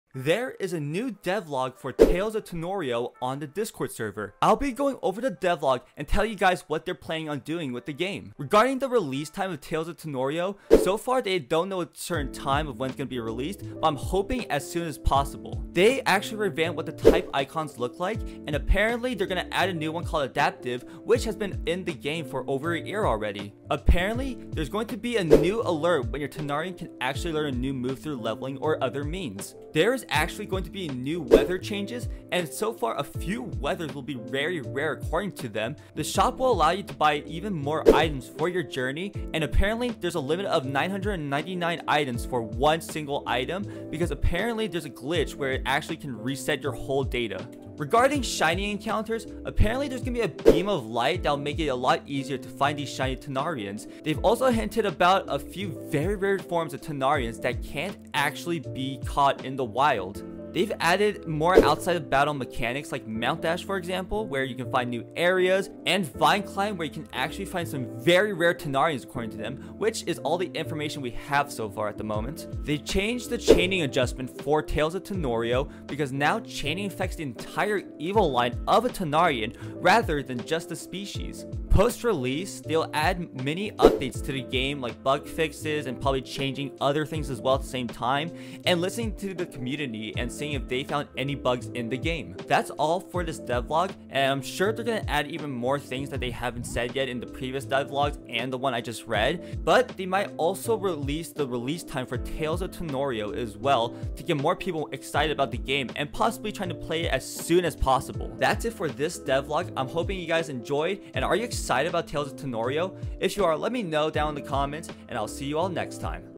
The NEWEST Dev Log for Tales of Tanorio
In this video, I will be talking about the newest dev log that got released about Tales of Tanorio. There is some new information regarding the game and I'm sure that they will add even more thins in the future.